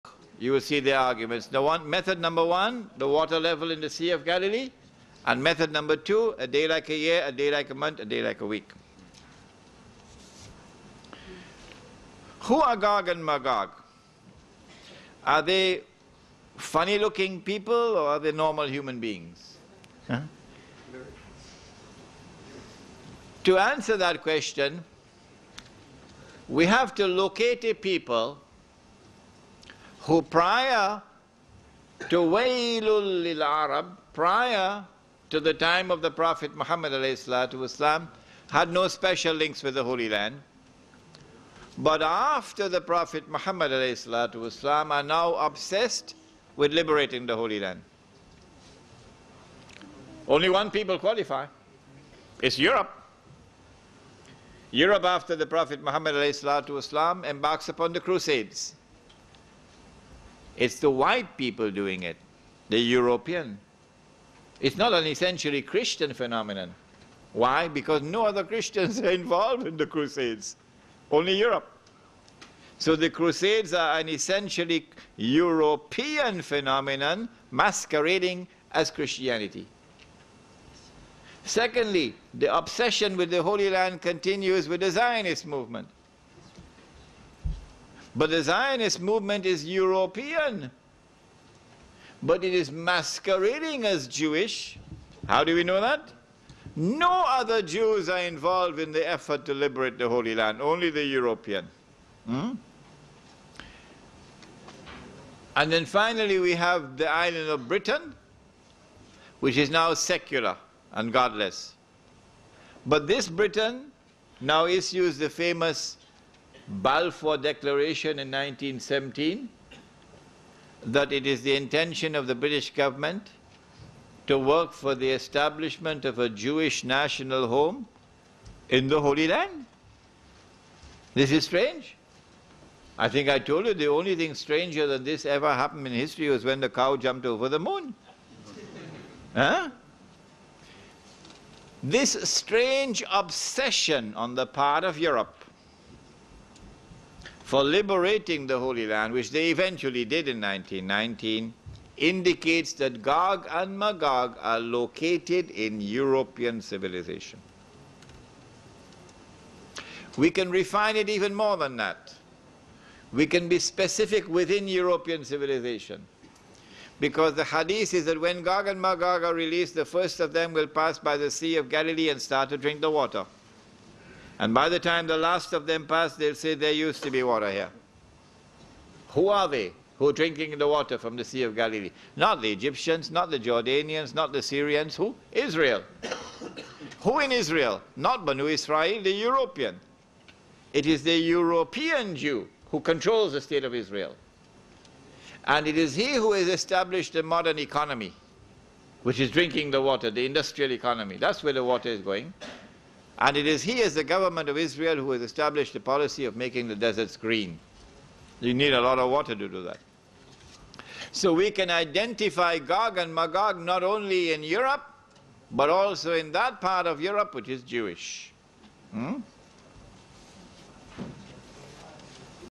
Who are Gog (Ya'juj) n Magog (Ma'juj) by Sheikh Imran Hosein
Sheikh Imran Hosein answering a question regarding who are the Gog and Magog. Many scholars have different opinion about the release of Gog and Magog, this is solely his interpretation using Quran and Authentic Hadis and connects the dots with current world affairs.

Sheikh uses following two verses from the Quran

He said: "This (wall) is a mercy from my Lord: but when the warning of my Lord comes to pass, He will reduce it to dust (and Gog and Magog would thus be released into the world); and the promise of my Lord is true."
(Qur'an, al-Kahf, 18:98)

and

"But there is a ban on a town which We have destroyed: that they (the people of the town) shall not return (to reclaim that town as their own); until Gog and Magog are let through (their barrier), and they swiftly spread out in every direction (replicating themselves amongst all the peoples of the world)."
(Qur'an, al-Anbiyah, 21:95-96)

Gog and Magog are described as two people who descended from father Adam (peace and blessings of Allah Most High be upon him) and who were created by Allah Most High, and endowed with great power. "None but I can destroy them", said Allah (Sahih Muslim :: Book 41 : Hadith 7016 ).

Research yourself, discuss with sheikhs, try to get explanation of those verses of Quran and try to analyze with current world affairs.

Allah says, This Quran may explain all things.